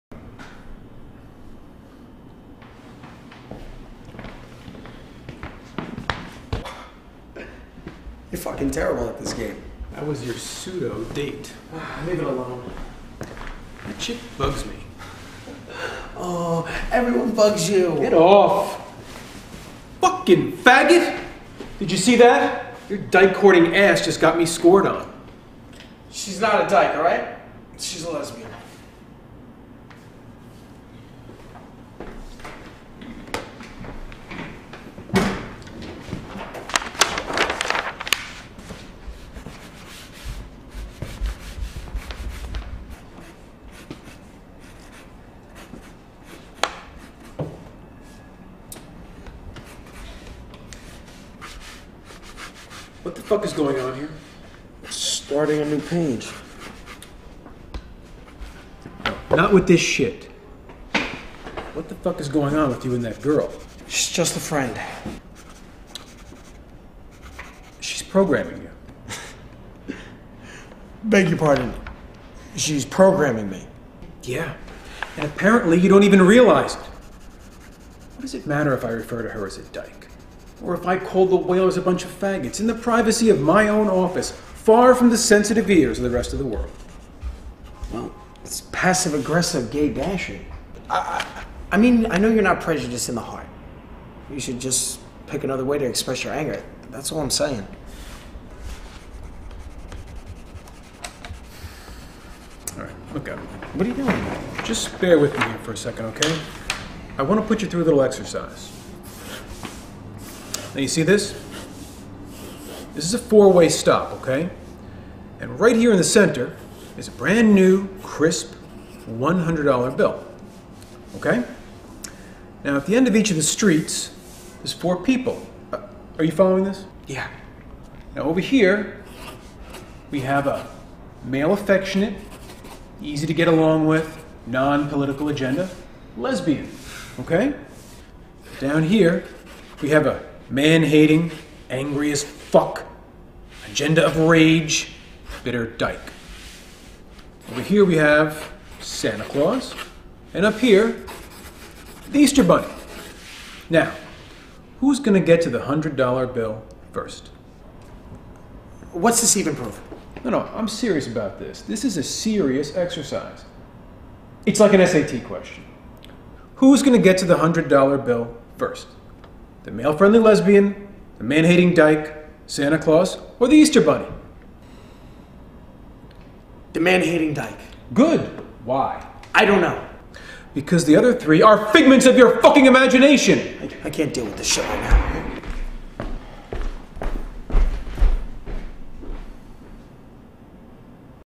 Scene from "Chasing Amy"
This is a scene from the movie "Chasing Amy" I did for a showcase at Weist Barron Studios. I play the part of "Banky"